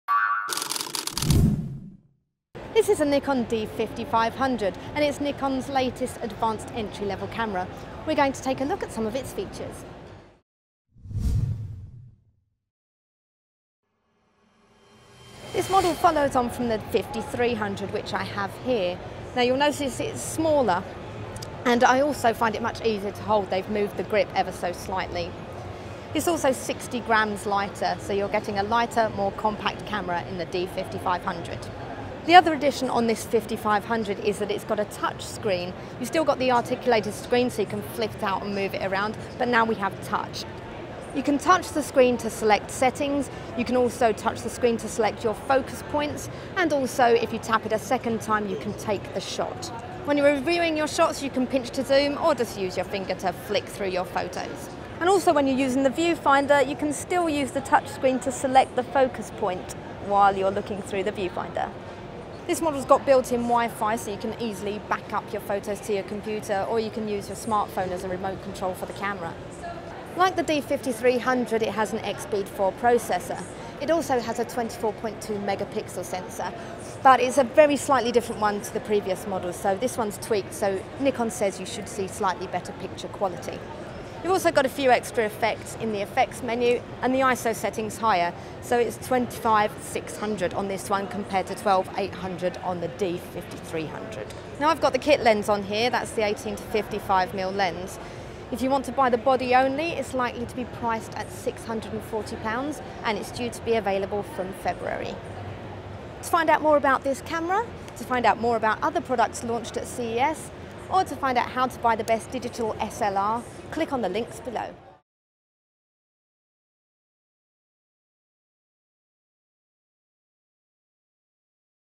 Nikon D5500 - Which first look from CES 2015
Nikon has announced the Nikon D5500 – a mid-level DSLR range with a built in touchscreen.

The Nikon D5500 isn’t so much a leap ahead as a little lift from what’s gone before. It replaces the D5300 and, although the processor on both models is the same, the 24.4Mp CMOS sensor on the new D5500 has received a few tweaks which Nikon says should be enhance the quality of your final shots.